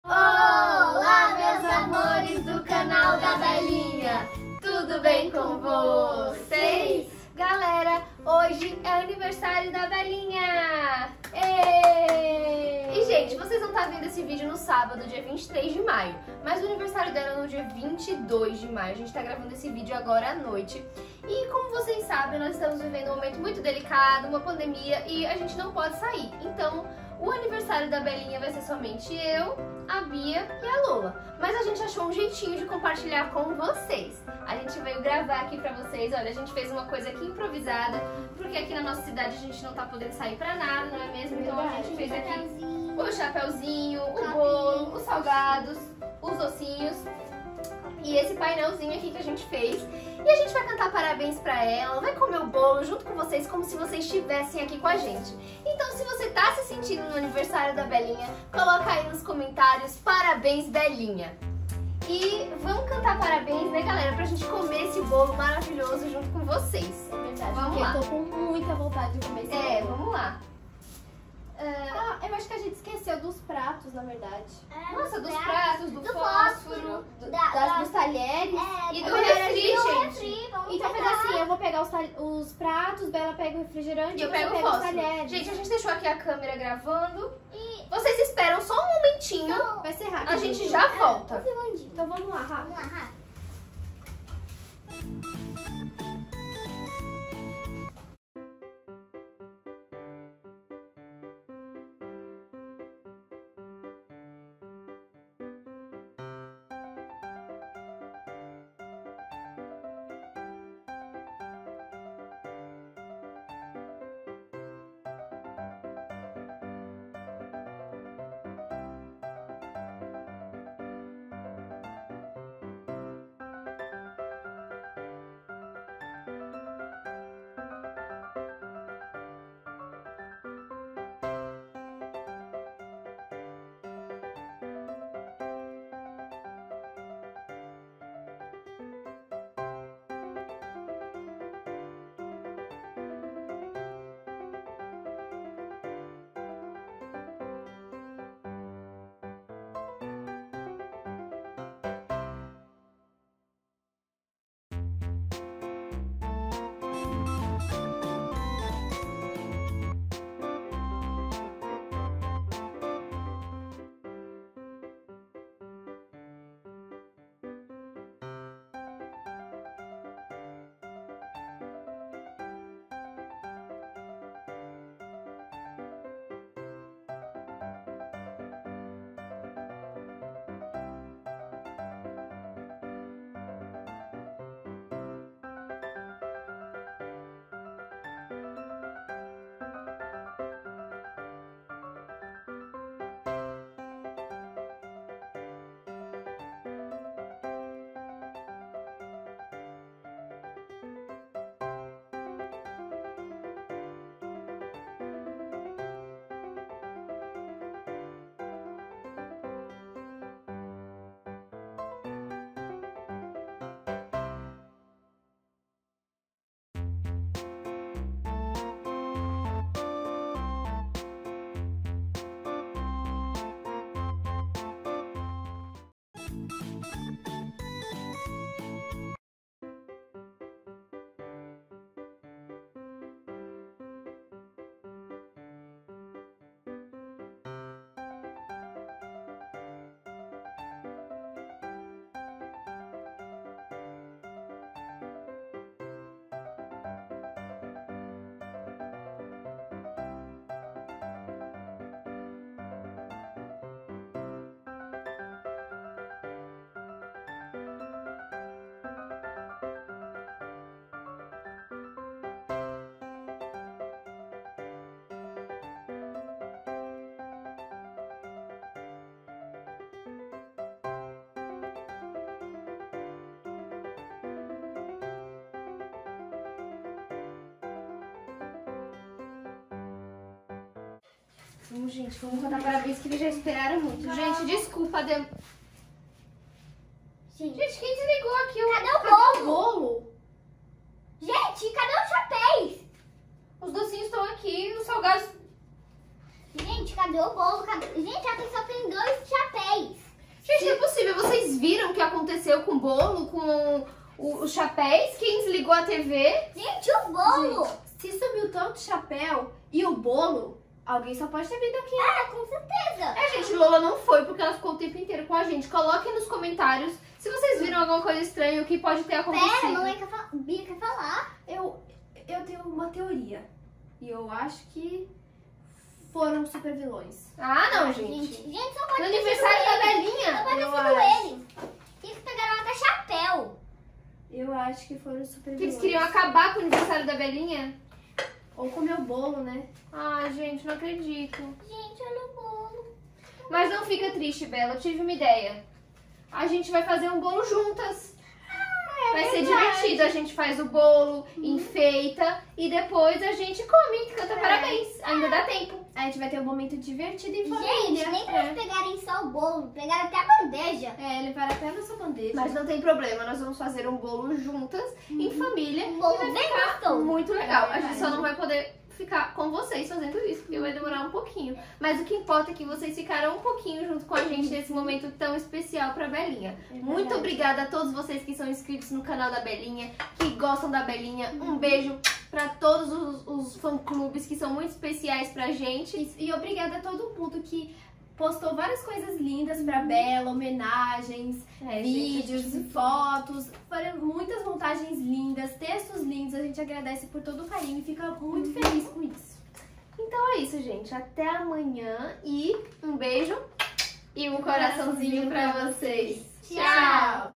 O IT A COISA,O ZAP E OS SUPER VILÕES INVADIRAM O ANIVERSÁRIO DA BELINHA
No vídeo de hoje, Belinha, Lua e Bia estão comemorando o aniversário da Belinha, mas algo estranho aconteceu. Roubaram o bolo da Belinha. Será que os super vilões IT A COISA, palhaço do mal PENNYWISE, Samara Morgan, Freddy Krueger, Zap e Chucky são os culpados?
E não se esqueça de me seguir no INSTAGRAM: @canaldabelinha
E também no TIKTOK: @canaldabelinhaoficial